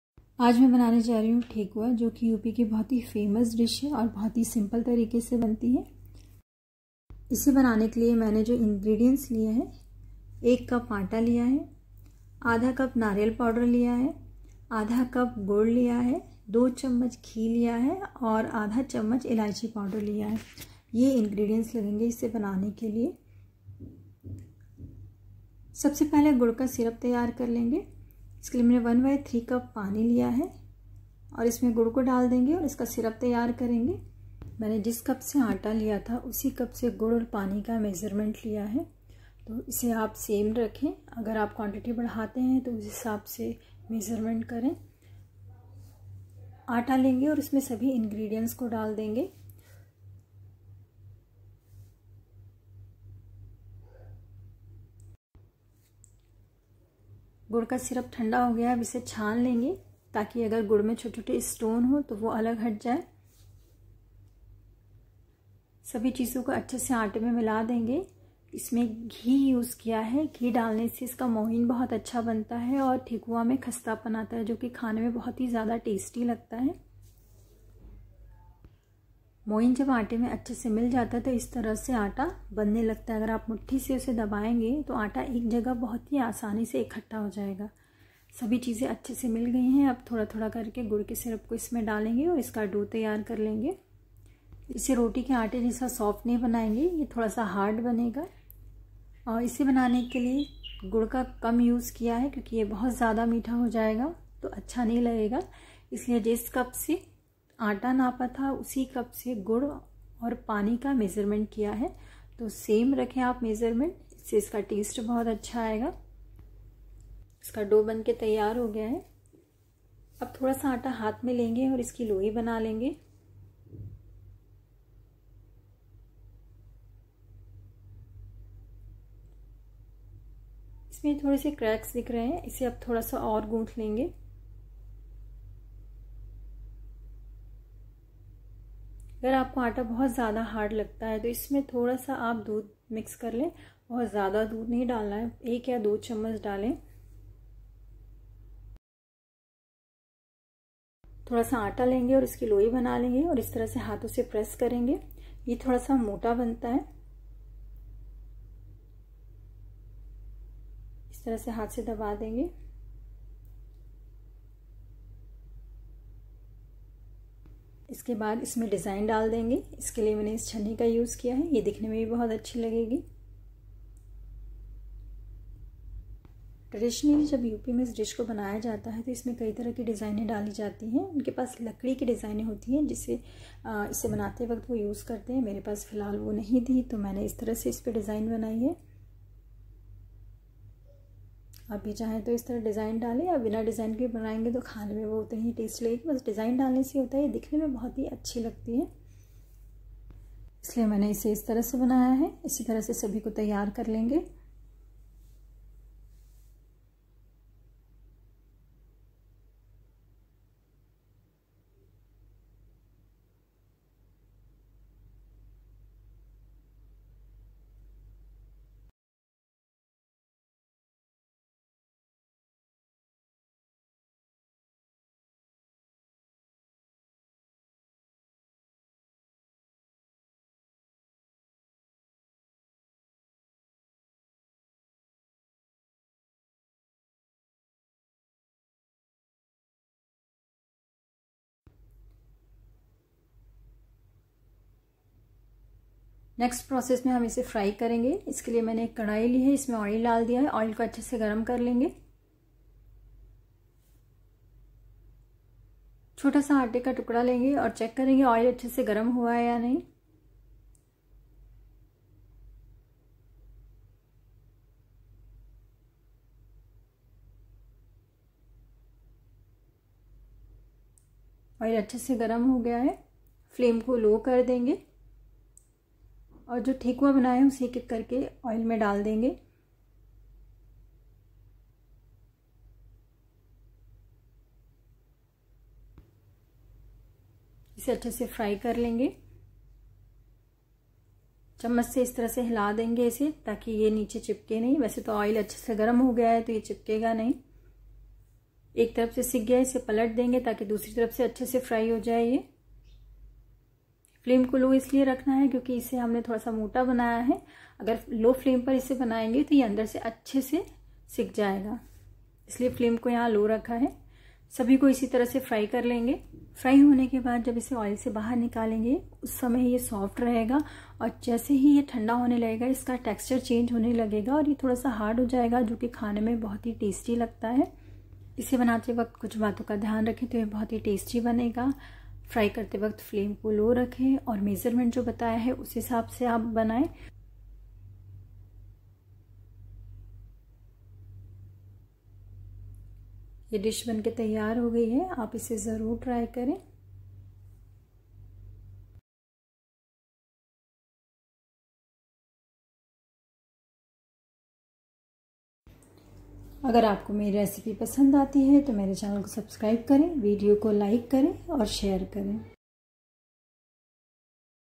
!!Thekua!! खस्ता ठेकुआ !!
Ingredients

1 cup wheat flour
Oil
1/3 cup water
1/2 cup Coconut powder
1/2 cup jaggery
2 tbsp ghee
1/2 tbsp elaichi Powder